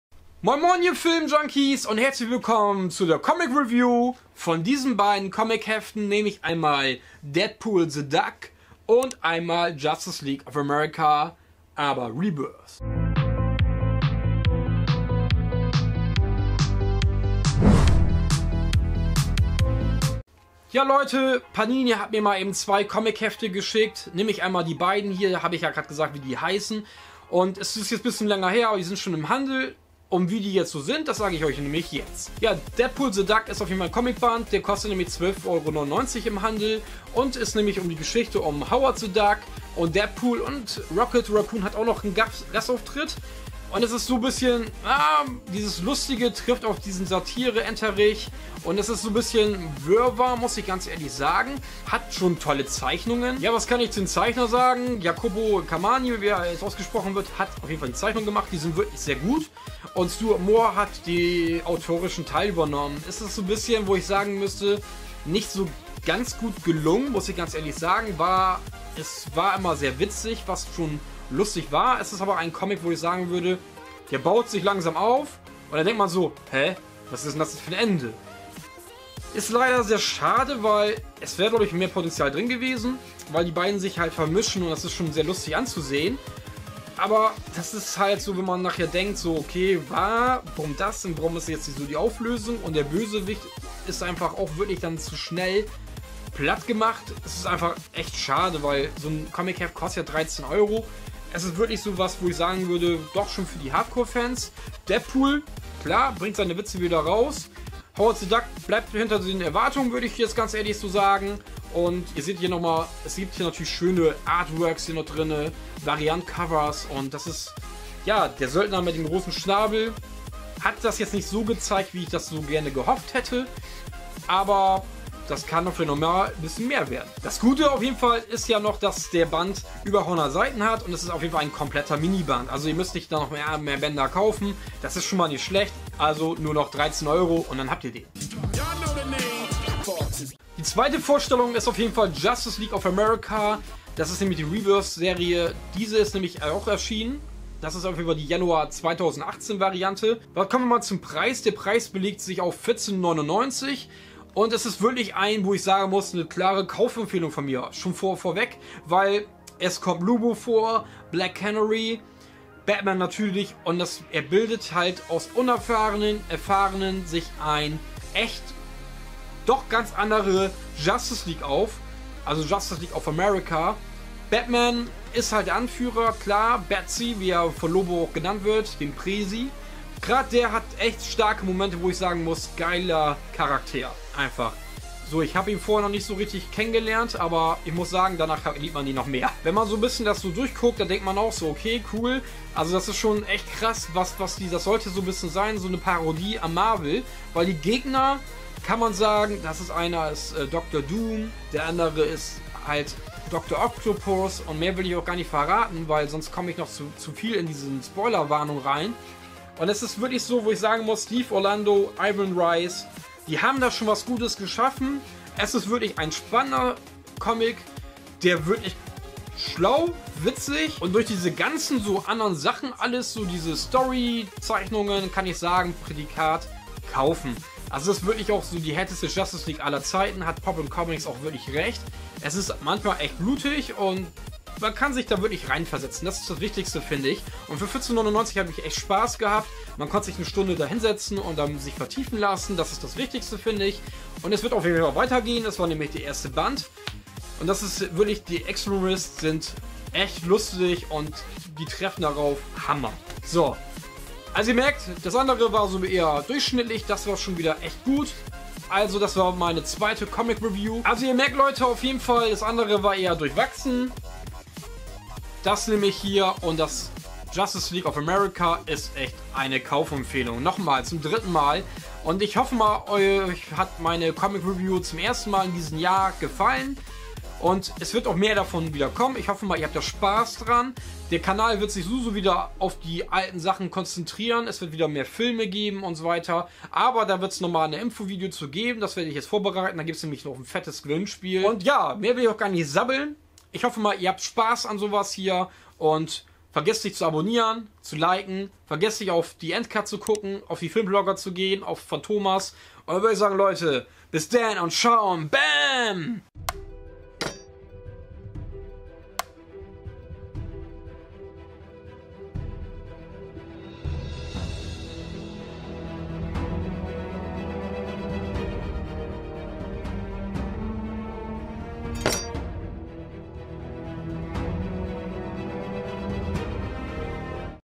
Comic Softcover Review - Deadpool the Duck + Justice League of America 1 Rebirth - Extremists
Comic Review zwei in eins - Beide mit Softcover in einem Video vereint! German deutsch. DEADPOOL THE DUCK - DER SÖLDNER MIT DEM GROSSEN SCHNABEL - SOFTCOVER + JUSTICE LEAGUE OF AMERICA 1: DIE EXTREMISTS - SOFTCOVER

JUSTICE LEAGUE OF AMERICA 1: DIE EXTREMISTS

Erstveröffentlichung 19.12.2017
Format: Softcover
Seiten: 132
Autoren: Steve Orlando
Zeichner: Ivan Reis
Storys: Justice League of America 1-6

Der Dunkle Ritter Batman führt ein neues Justice League of America-Team auf seine erste Mission. Doch ist diese ungewöhnliche JLA schon bereit für einen Kampf mit ebenso mächtigen wie skrupellosen kosmischen Extremisten? Das hängt davon ab, wie gut die Truppe aus Anti- und Nachwuchshelden zusammenarbeitet.

Die erste Mission der neuen JLA!

Preis: 14,99€ im Handel

DEADPOOL THE DUCK - DER SÖLDNER MIT DEM GROSSEN SCHNABEL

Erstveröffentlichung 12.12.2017
Format Softcover
Seiten 116
Autoren Stuart Moore
Zeichner Jacopo Camagni
Storys Deadpool the Duck 1-5

Als Rocket Raccoon durchdreht, soll Deadpool ihn für SHIELD dingfest machen. Der vom Pech verfolgte Howard kommt eher durch Zufall vorbei, und ein noch größerer Zufall sorgt dafür, dass Mr. Wilson und Mr. Duck verschmelzen! Doch wer hat die Kontrolle über den Körper der Ente, und wird Deadpool the Duck die bevorstehende Weltraum-Mission überleben?

Komplette Serie mit der Ente mit der großen Klappe in einem Band!
Für alle Fans von DEADPOOL, ROCKET RACCOON & HOWARD THE DUCK!

unbezahlte Produktplatzierung, aber ein Dank an Panini Comics! oder Aber auch zu finden auf YouTube, Facebook usw.